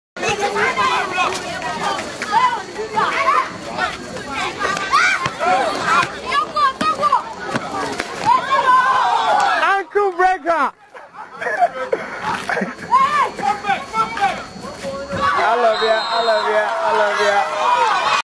Kids ballin
yes, kids balling too in Ghana. here we see participants of the Hoops Care International basketball clinic in their first match. These kids are improving at a faster rate than expected.